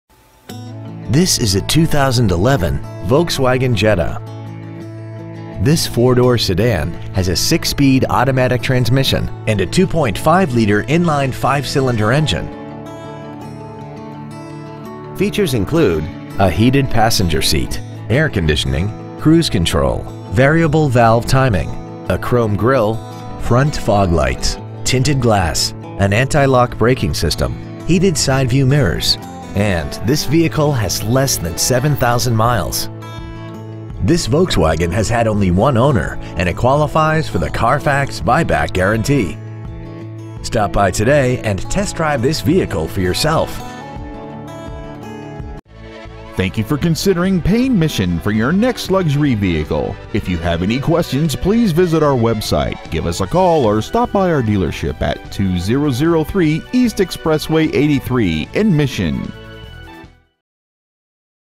2011 Volkswagen Jetta Laredo TX
Year : 2011
 Make : Volkswagen
 Model : Jetta
 Engine : 2.5L 5 cyls
 Trans . : Automatic 6-Speed
 Exterior : Platinum Gray Metallic
 Miles : 6,474
 Interior : Titan Black w/Titan Black V-Tex Leatherette Seat T
 Stock : T353431

 2003 E. Expressway 83